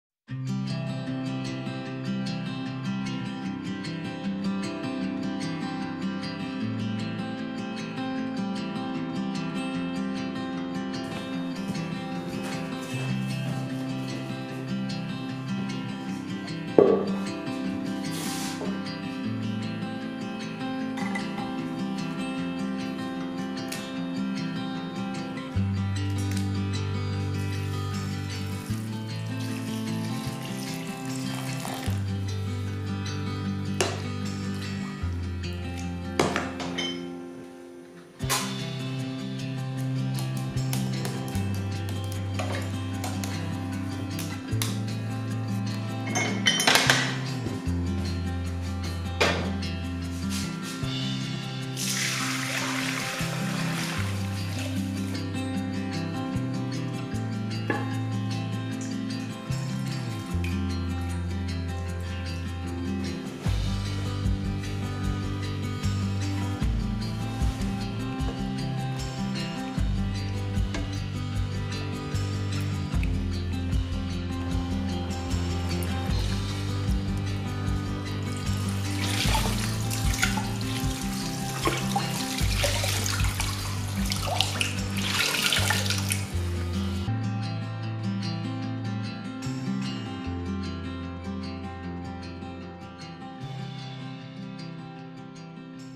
Teinture à la tanaisie
Pour réaliser ce projet de teintre des serviettes de table avec de la tanaisie (Tanacetum vulgare) je ne voulais pas utiliser de mordant pour le faire le plus naturellement et simplement avec ce que j'avais dans ma cuisine.
Après séchage on obtient un jaune pastel.